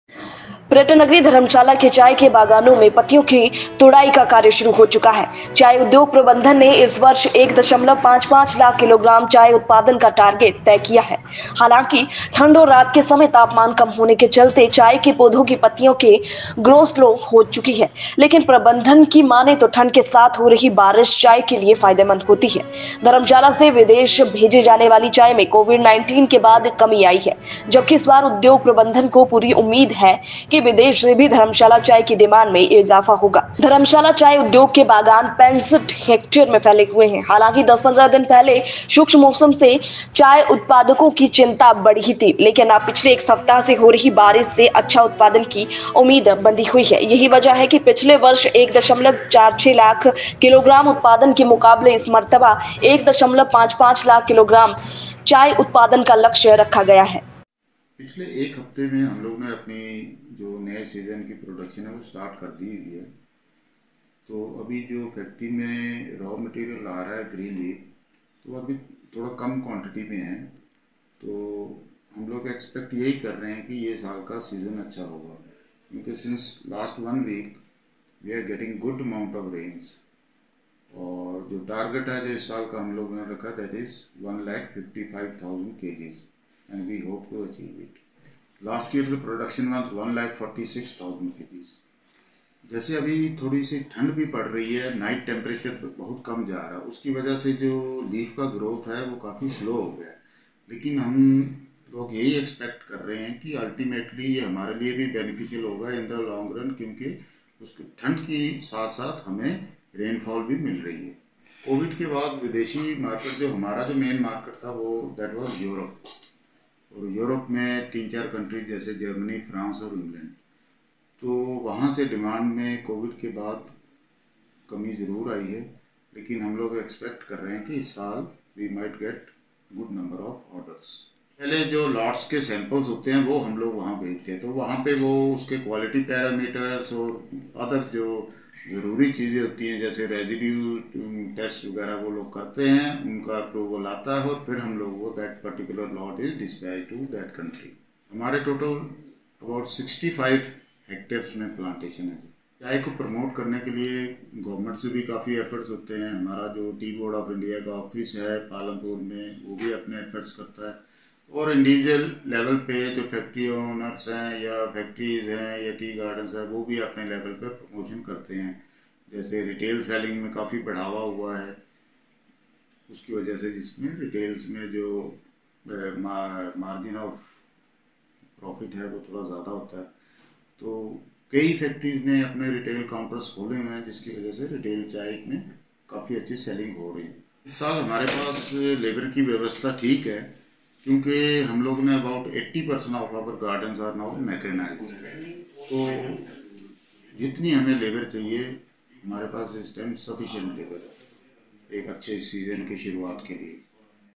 Dharamshala | Tea Production | Tea Gardens
Plucking of leaves has started in the tea gardens of tourist town Dharamshala. The Tea Industry Management has set a target of 1.55 lakh kg tea production this year.